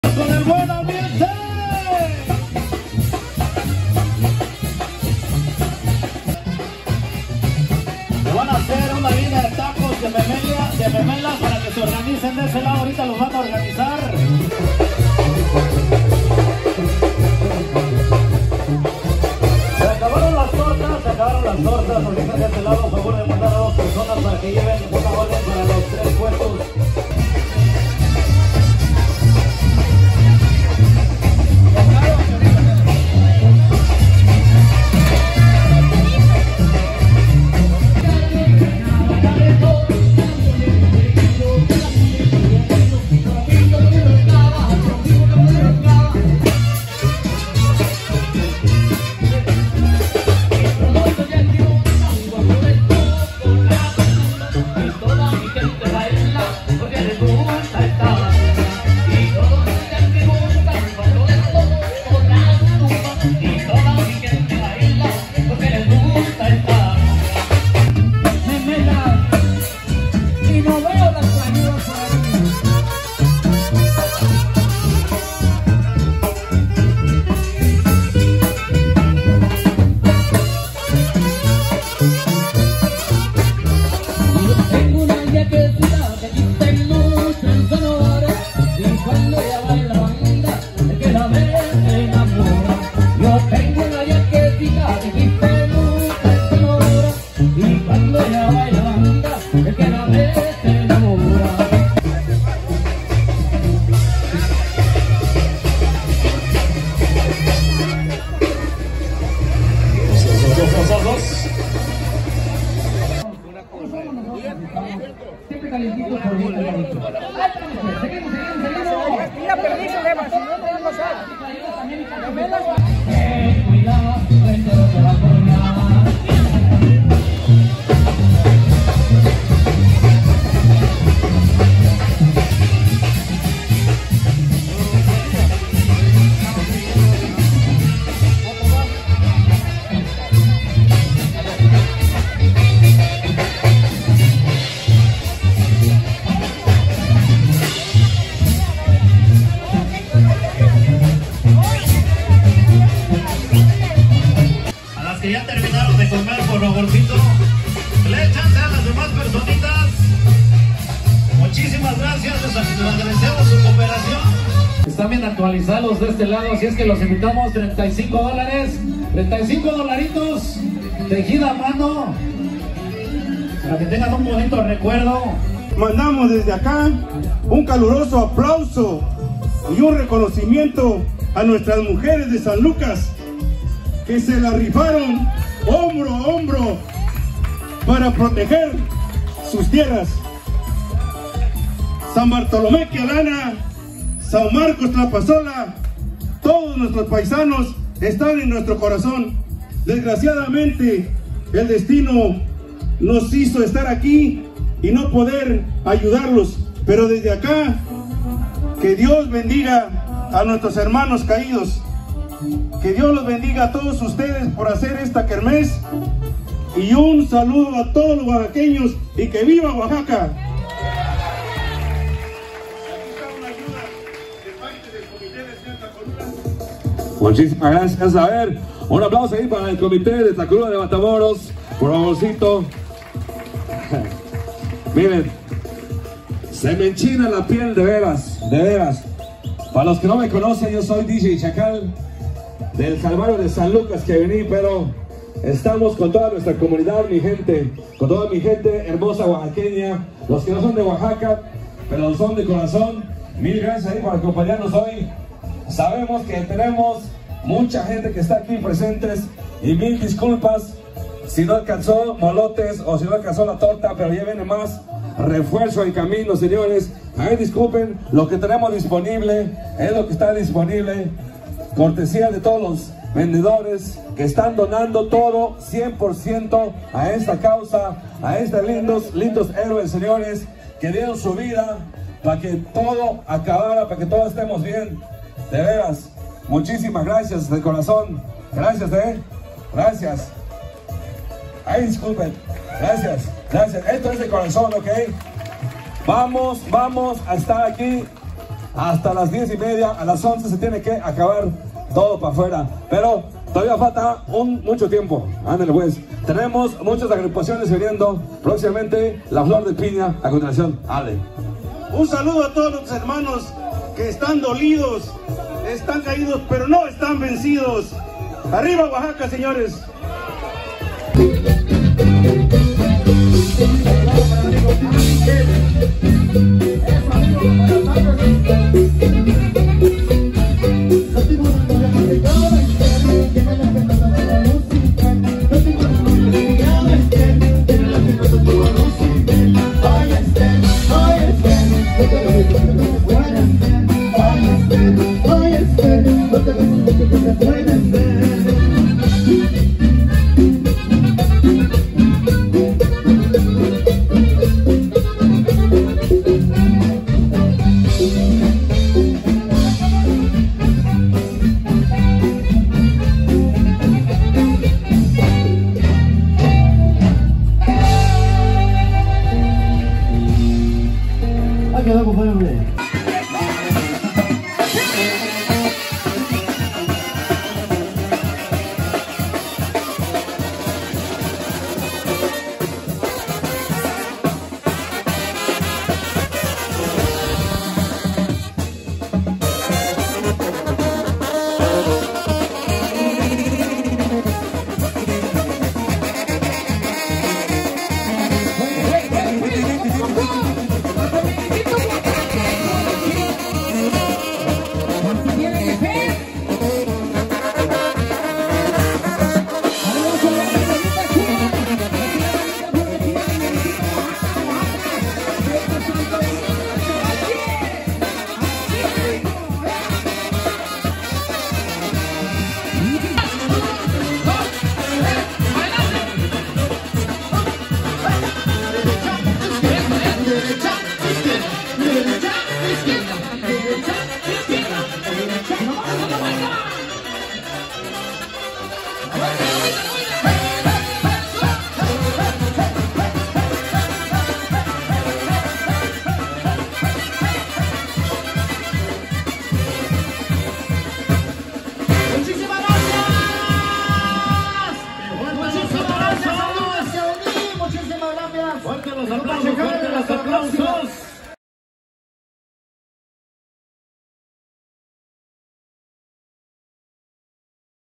KERMÉS Por San Lucas Quiavini 2024
Kermés en honor a los héroes caídos en los incendios de san lucas Quiavini oaxaca